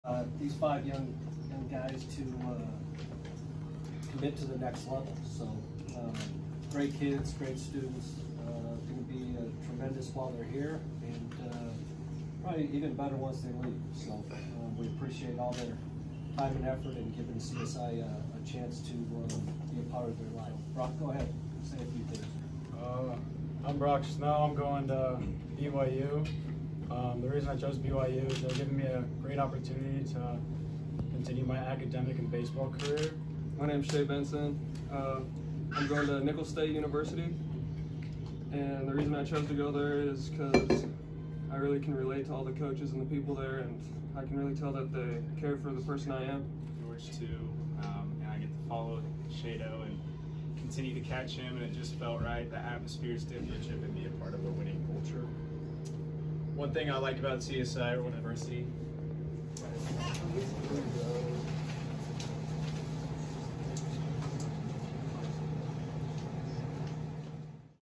5 CSI baseball players signing with four-year schools
College of Southern Idaho sophomores Shay Benson (Nicholls State), Brexten Starley (Nicholls State), Coleman Bobb (VCU), Brock Snow (BYU) and Jack Barker (Colorado Mesa) signed to their next school.